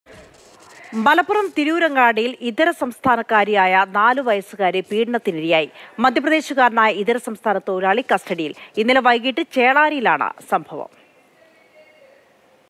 തിരൂരങ്ങാടിയിൽ നാല് വയസ്സുകാരി പീഡനത്തിനിരയായി | crime
ഇതര സംസ്ഥാനത്തൊഴിലാളി പിടിയിൽ

Happy viewing!